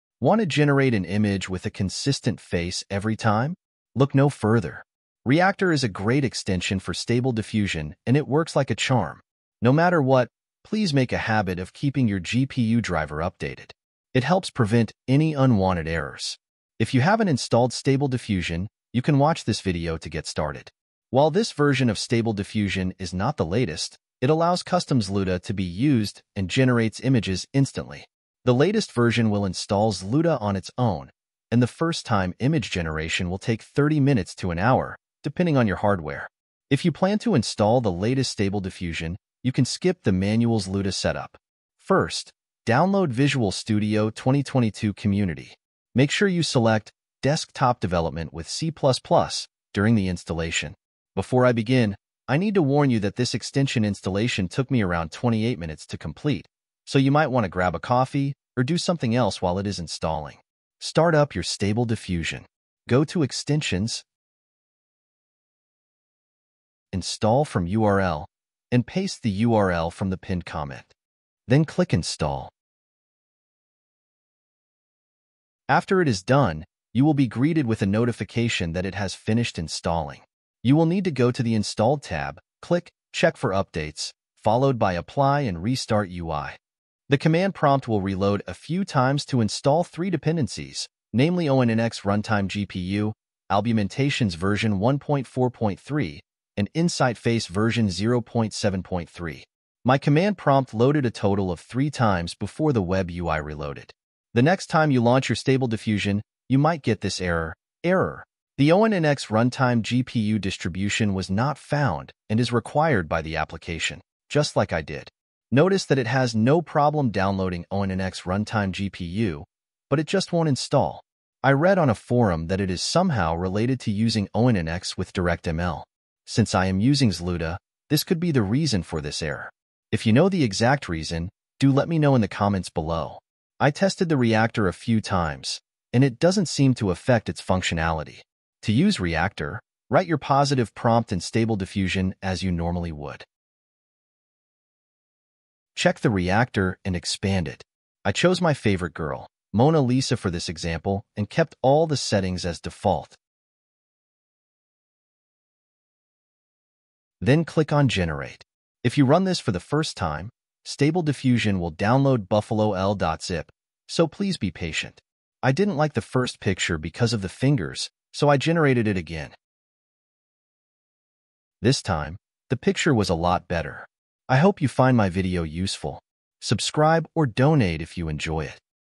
Gourieff/sd-webui-reactor: Install ReActor For Stable Diffusion with ZLUDA ft. Mona Lisa
UPDATE 10 June 2024:
Please do not attempt to fix onnxruntime-gpu by manual installing it if you are you using stable diffusion with zluda, it WILL BREAK EVERYTHING

0:00 - Intro
0:09 - Update Your AMD GPU Driver
0:17 - Using Reactor with Stable Diffusion ZLUDA for AMD GPU
0:41 - Install and Setup Visual Studio 2022
0:51 - Warning
1:01 - Installing Reactor
1:43 - Notable Error
2:18 - Reactor Usage Demo

Want to generate an image with a consistent face every time? Look no further. Reactor is a great extension for Stable Diffusion and it works like a charm.

No matter what, please make a habit of keeping your GPU driver updated. It helps prevent any unwanted errors.

If you haven't installed Stable Diffusion, you can watch this video to get started. While this version of Stable Diffusion is not the latest, it allows custom ZLUDA to be used and generates images instantly. The latest version will install ZLUDA on its own, and the first-time image generation will take 30 minutes to an hour, depending on your hardware. If you plan to install the latest Stable Diffusion, you can skip the manual ZLUDA setup.

First, download Visual Studio 2022 Community. Make sure you select "Desktop Development with C++" during the installation.

Before I begin, I need to warn you that this extension installation took me around 28 minutes to complete, so you might want to grab a coffee or do something else while it is installing. Start up your Stable Diffusion. Go to Extensions, Install from URL, and paste the URL from the pinned comment. Then click install.

After it is done, you will be greeted with a notification that it has finished installing. You will need to go to the Installed tab, click "Check for updates," followed by "Apply and restart UI." The command prompt will reload a few times to install three dependencies, namely onnxruntime-gpu, albumentations version 1.4.3, and insightface version 0.7.3. My command prompt loaded a total of three times before the web UI reloaded.

The next time you launch your Stable Diffusion, you might get this error: "Error: The 'onnxruntime-gpu' distribution was not found and is required by the application," just like I did. Notice that it has no problem downloading onnxruntime-gpu, but it just won't install. I read on a forum that it is somehow related to using ONNX with DirectML. Since I am using ZLUDA, this could be the reason for this error. I tested the Reactor a few times, and it doesn't seem to affect its functionality.

To use Reactor, write your positive prompt in Stable Diffusion as you normally would. Check the Reactor and expand it. I chose my favorite girl, Mona Lisa, for this example and kept all the settings as default. Then click on Generate. If you run this for the first time, Stable Diffusion will download buffalo_l.zip, so please be patient. I didn't like the first picture because of the fingers, so I generated it again. This time, the picture was a lot better. I hope you find my video useful.